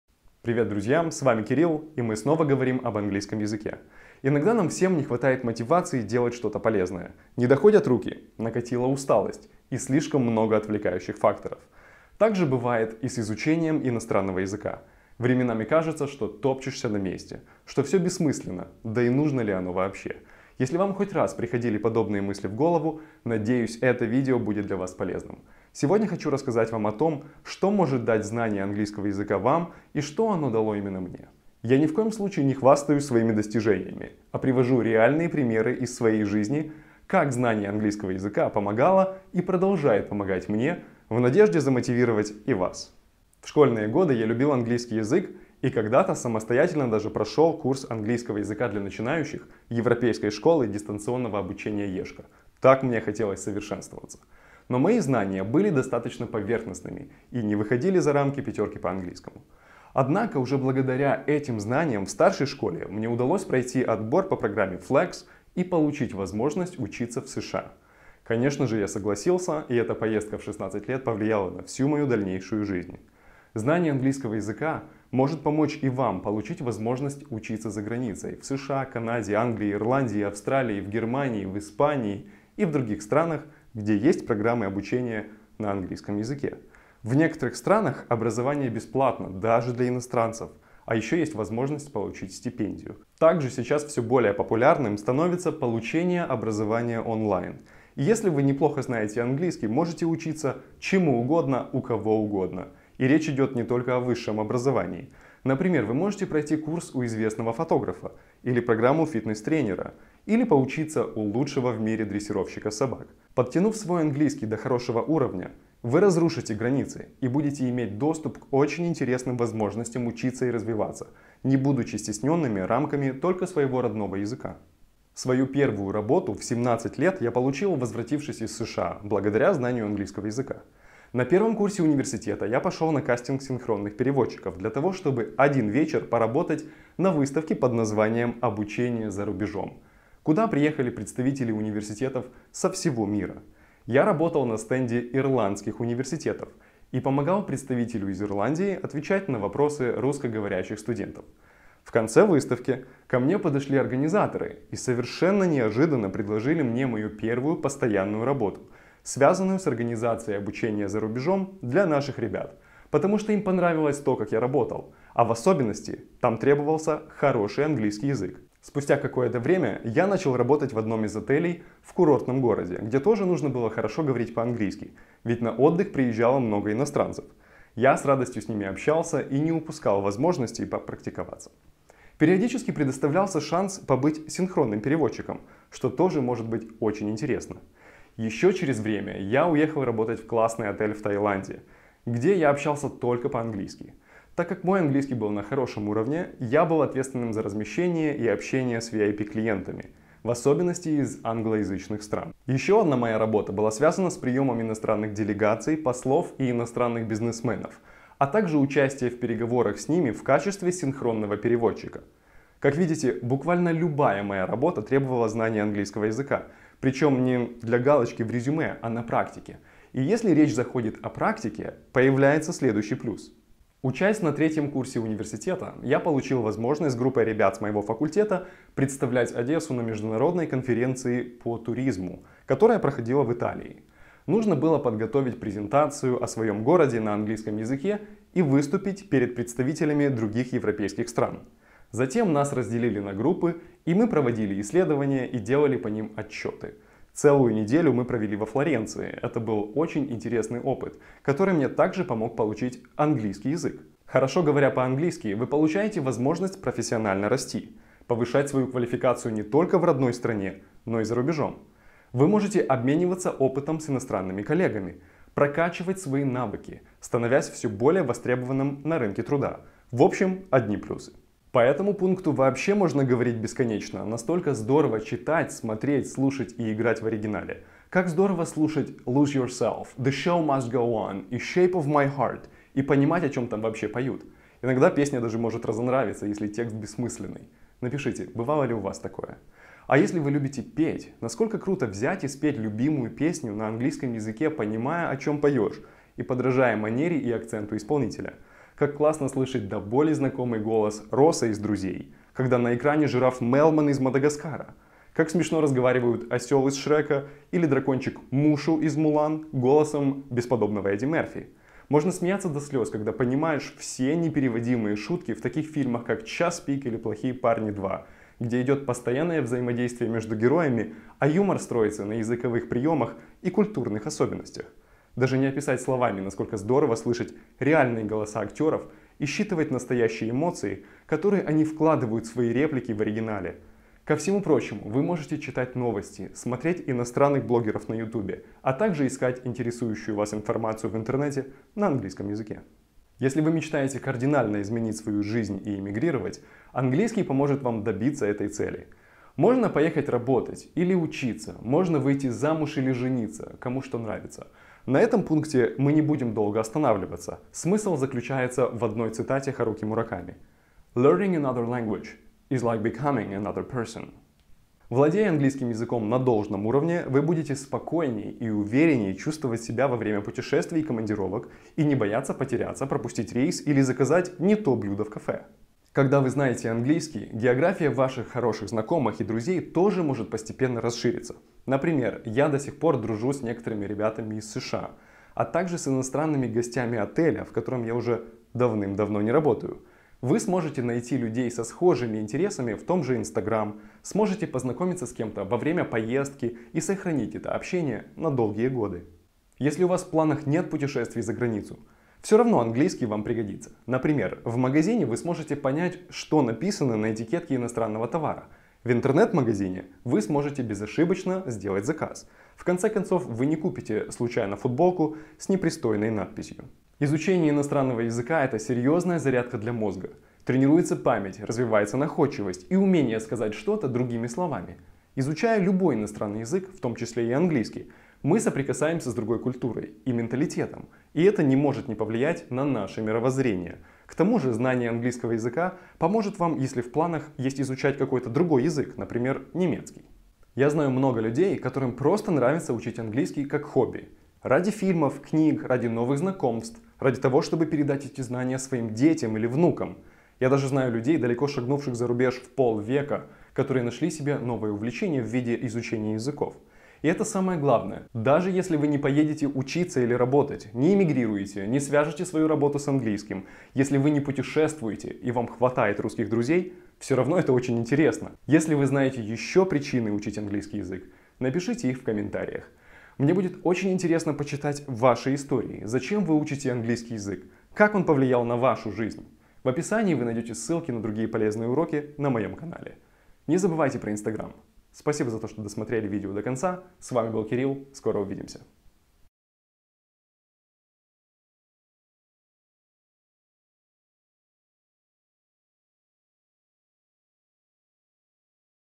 10 причин выучить английский язык
Иногда нам всем не хватает мотивации делать что-то полезное, не доходят руки, накатила усталость и слишком много отвлекающих факторов. То же бывает и с изучением иностранного языка, временами кажется, что топчешься на месте, что все бессмысленно, да и нужно ли оно вообще? Сегодня я хочу рассказать вам о том, что может дать знание английского вам и что оно дало именно мне. Я ни в коем случае не хвастаюсь своими «достижениями», а привожу реальные примеры из своей жизни, как знание английского языка помогало и продолжает помогать мне в жизни, в надежде замотивировать и вас.

Сказать спасибо:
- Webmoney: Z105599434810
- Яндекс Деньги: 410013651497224